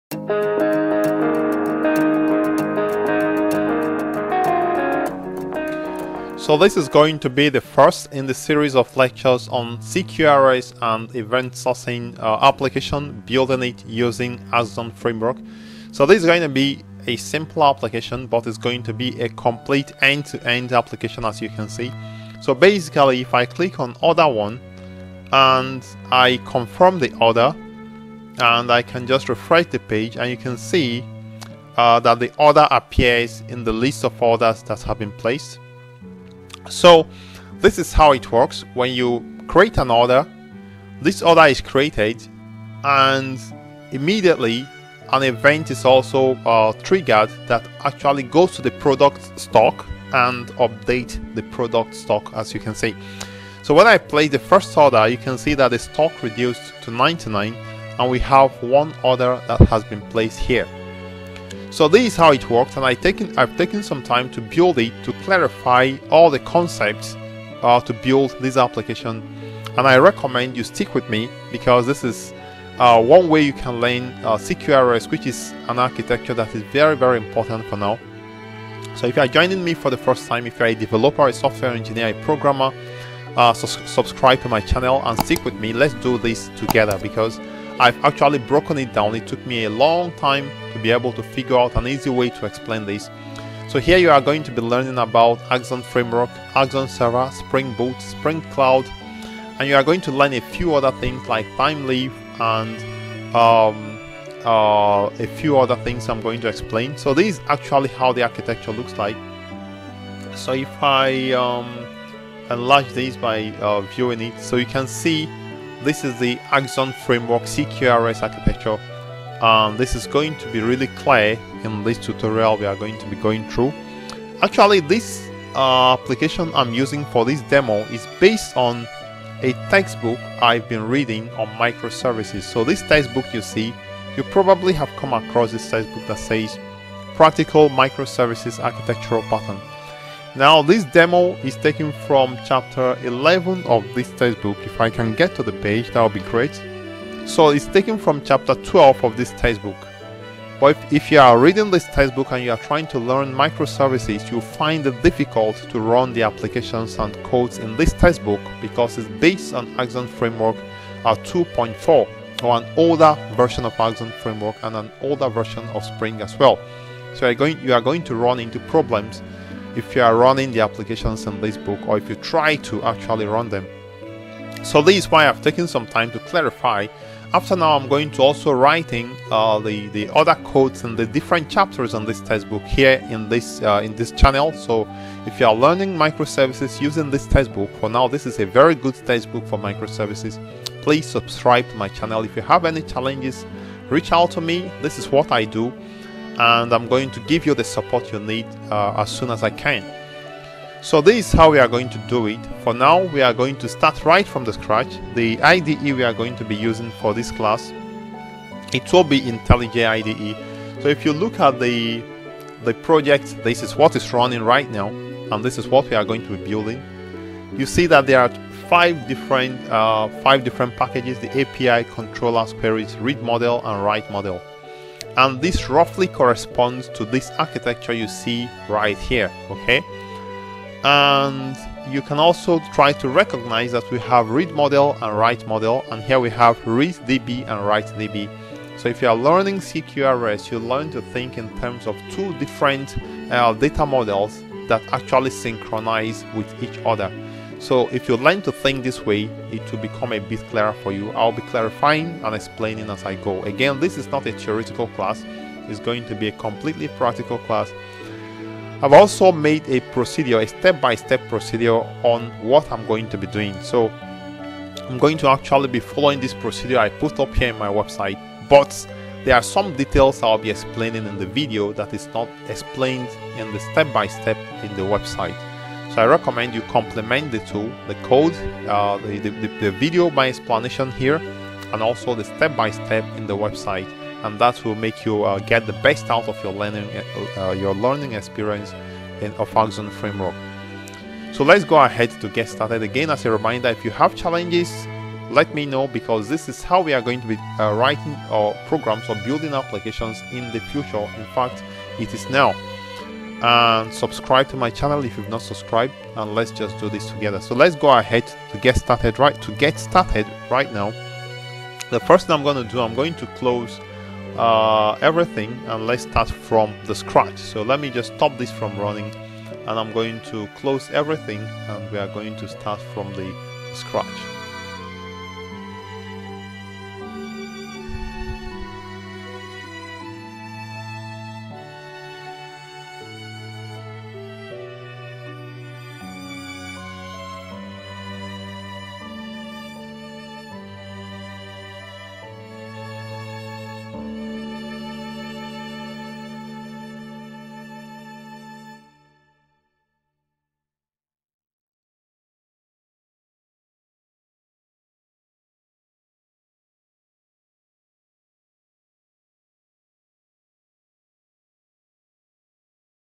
1 - Introduction to CQRS with Axon Framework - A Complete Application
This is Part 1 of a Step by Step Tutorial on the CQRS Pattern using Axon Framework.

In this tutorial, you will learn about:
👉The CQRS Pattern
👉The Axon Framework
👉Axon Server
👉Spring Framework/Spring Boot
👉Some HTML/CSS/JavaScript
👉Thymeleaf